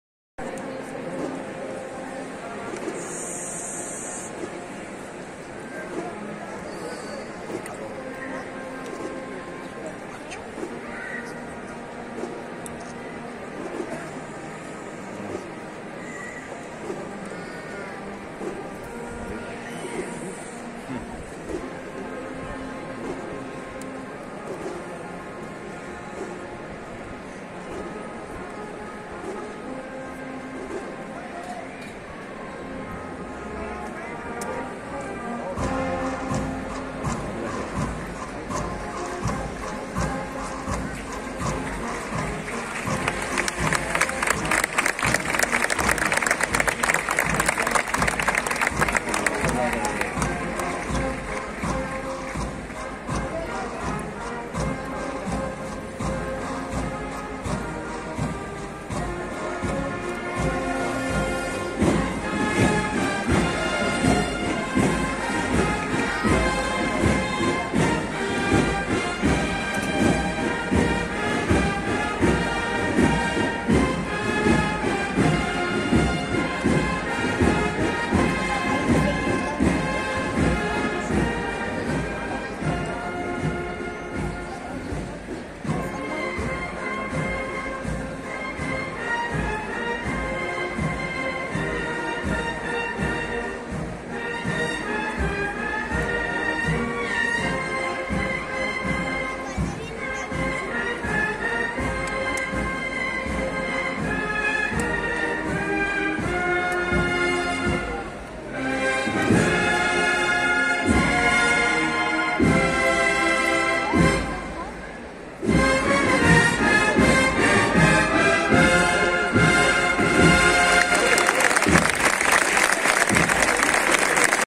Montesión en la Plaza del Duque 2022
Misterio de la Hermandad de Montesión en 2022 a su paso por la plaza del Duque acompañado musicalmente por la Agrupación Musical Redención de Sevilla